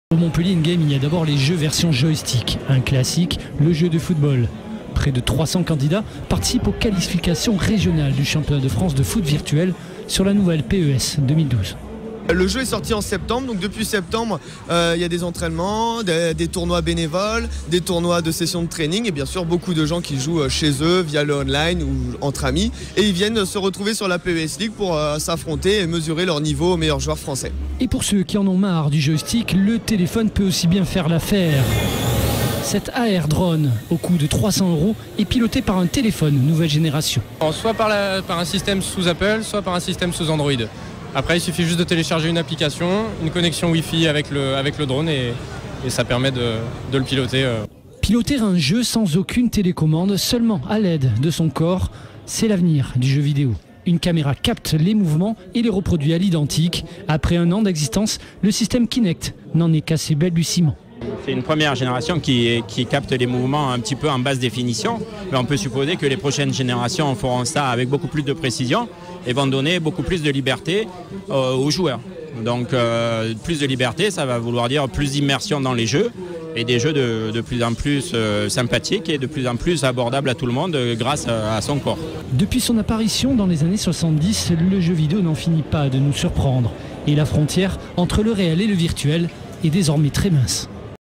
Montpellier In Game 2011
Sites webs/blogs, copiez cette vidéo avec le bouton "Intégrer" !
2ème édition du salon Montpelliérain consacré aux jeux vidéos. Pour exploitation TV, droits réservés, nous contacter